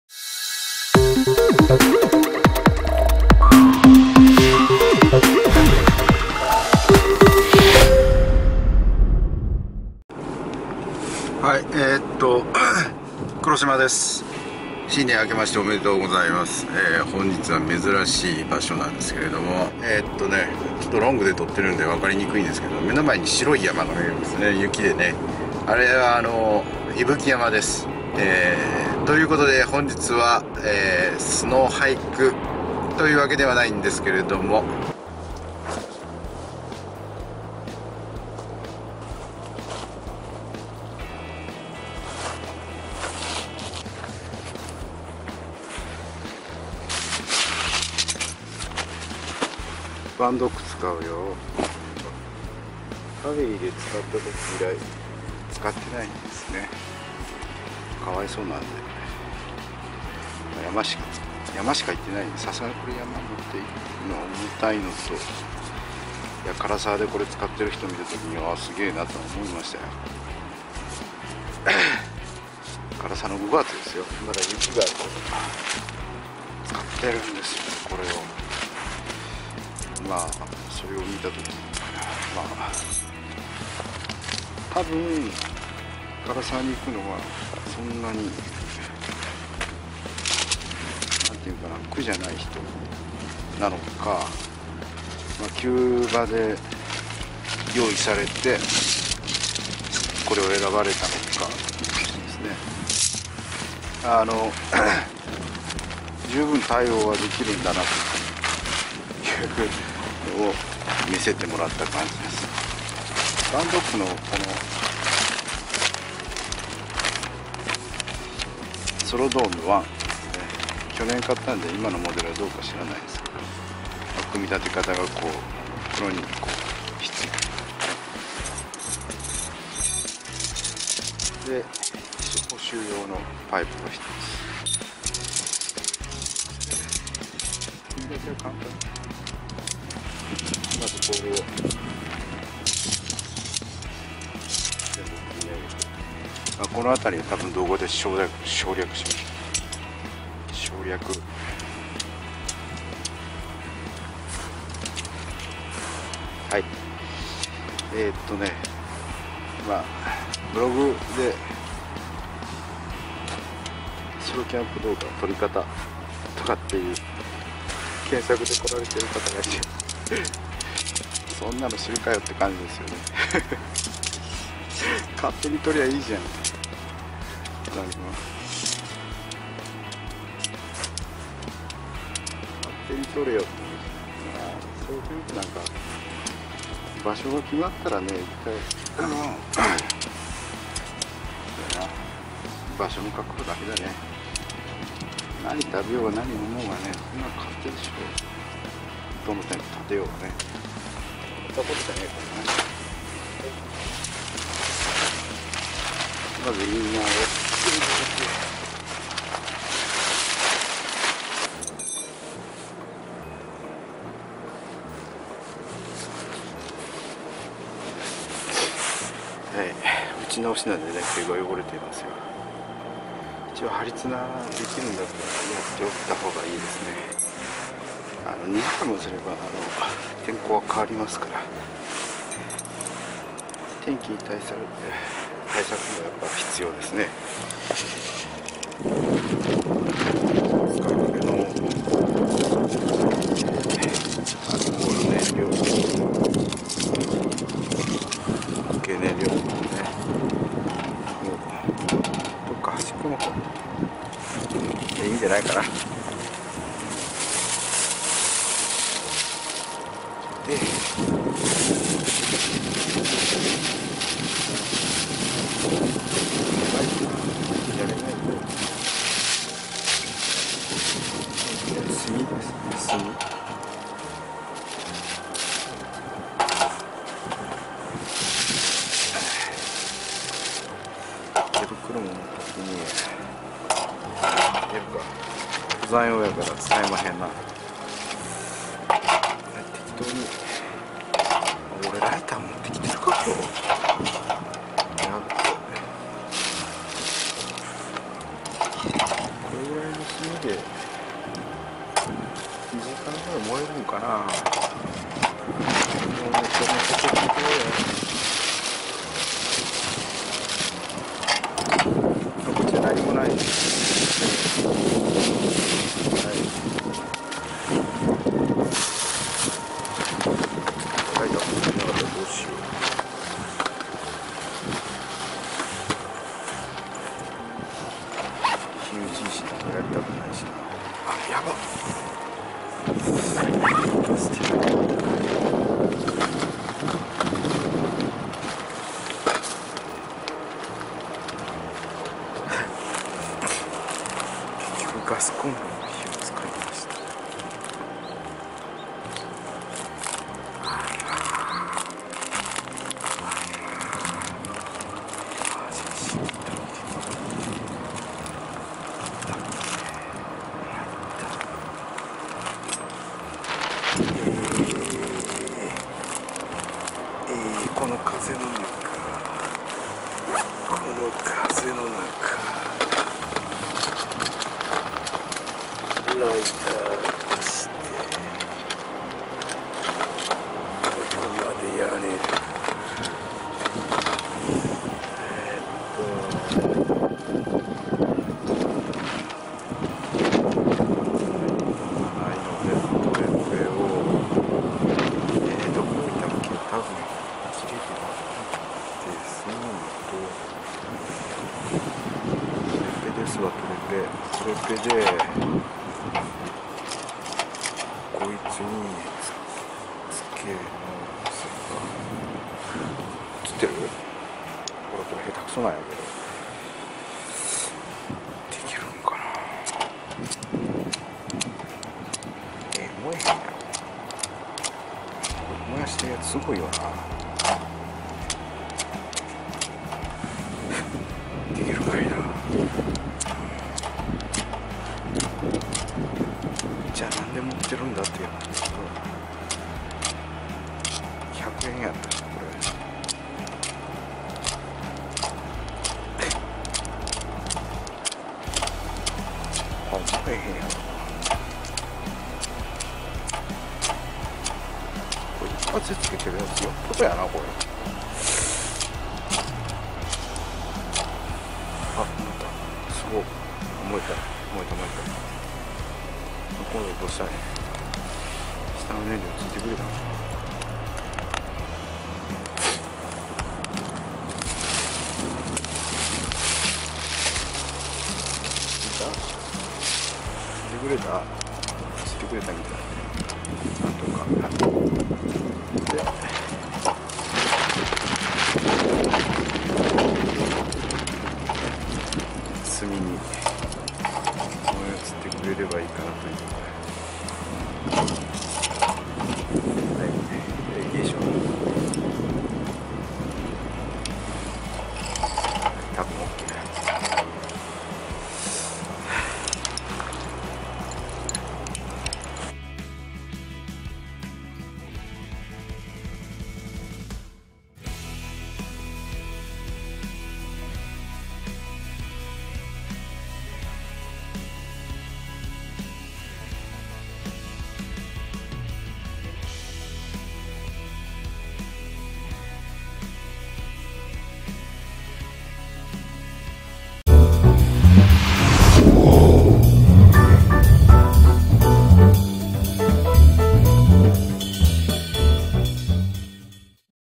粕川でソロキャンプ / 黒志摩修一
年末から腰痛で山に登れませんので道具を出して日向ぼっこのつもりでディキャンプ。どうせならと若いころに経験のある場所でキャンプ。